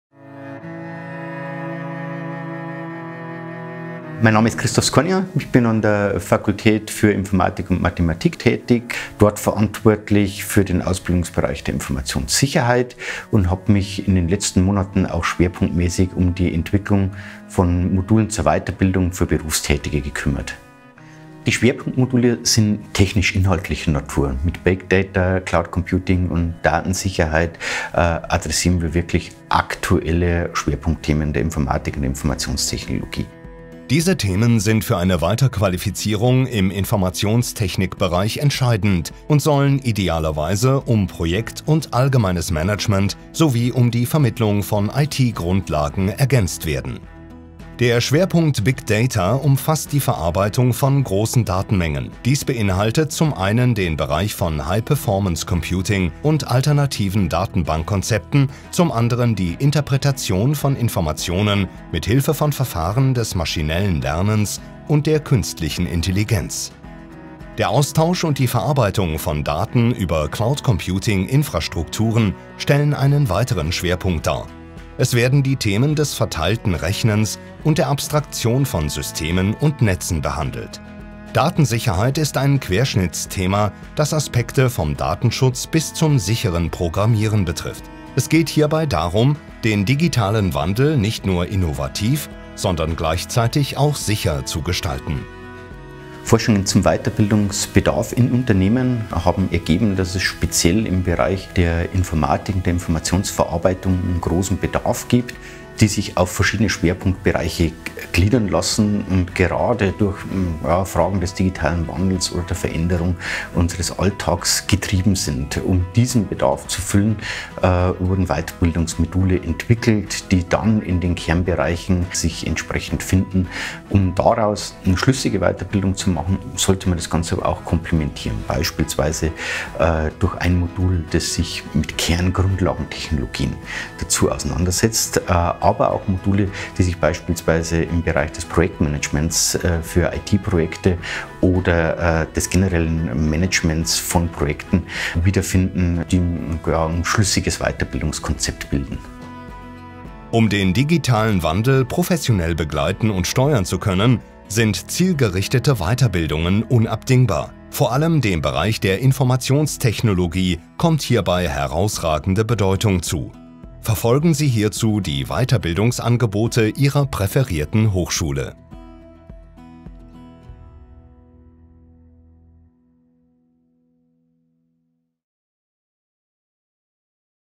Relevanz von Weiterbildungen in der Informationstechnologie
Der Informationsfilm verdeutlicht die Relevanz von Weiterbildungen in der Informationstechnologie, insbesondere vor dem Hintergrund des technologischen Wandels und der Digitalisierung. Das Video entstand im Rahmen des BMBF-Verbundprojektes OTH mind.
Inhalt: BMBF-Verbundprojekt OTH mind
Produktion: Oakstone Productions